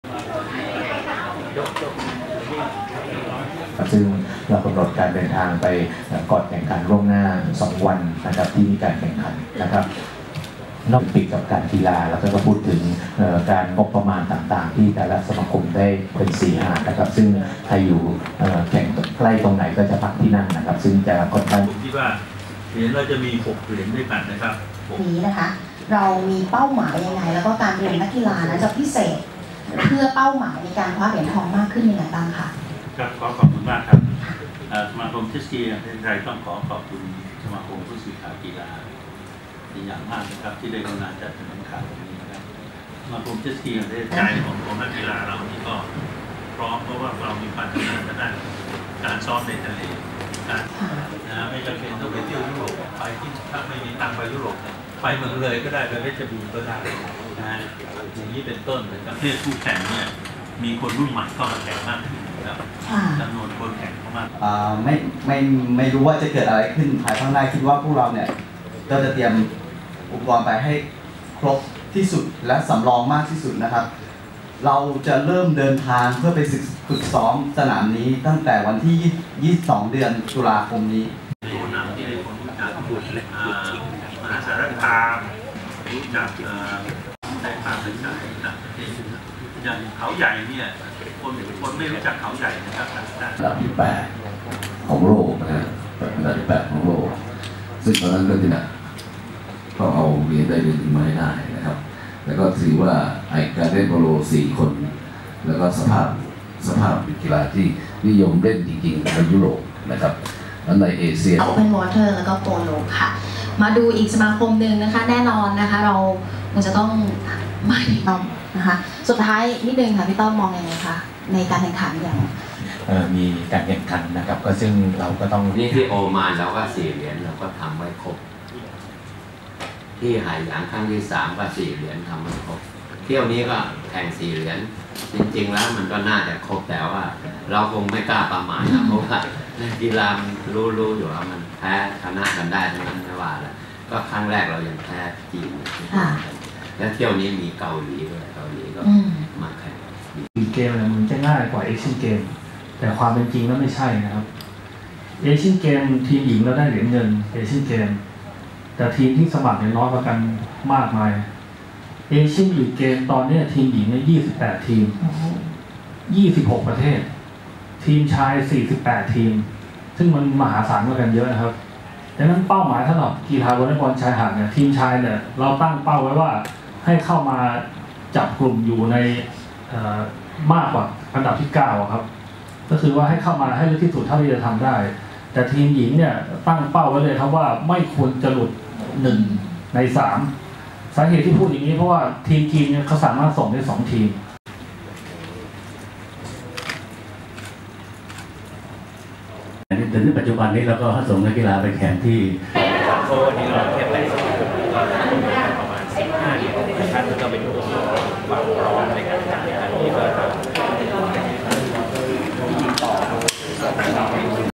สมาคมนักข่าวช่างภาพกีฬาแถลงข่าวความพร้อมเอเชี่ยนบีชเกมส์ครั้ง4,ภูเก็ต
สมาคมนักข่าวช่างภาพกีฬา ฯ จัดมีทเดอะเพรส แถลงข่าวความพร้อมทัพนักกีฬาเอเชี่ยนบีชเกมส์ครั้งที่ 4 จ.ภูเก็ต ณ ห้องศุภชลาศัย สนามกีฬาแห่งชาติปทุมวัน Photographer กิตตินันท์ รอดสุพรรณ (Kititnun Rodsupan)